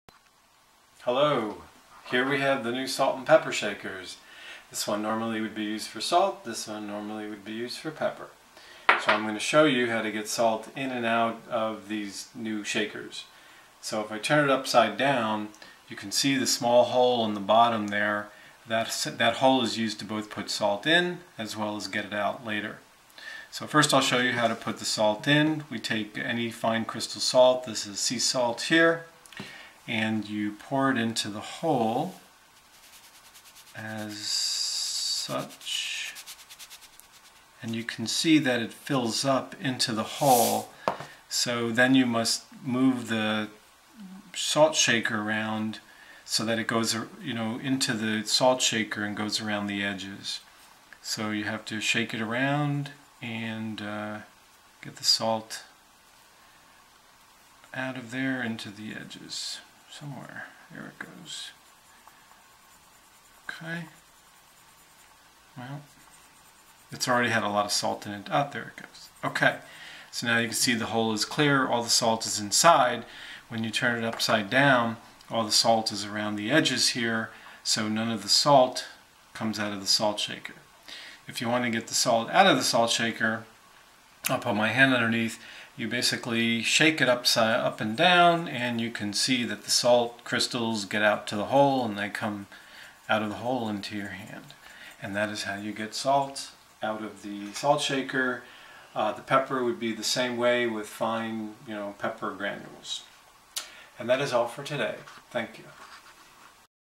Using the new salt & pepper shakers
Tutorial on how to fill and then use the new salt and pepper shakers.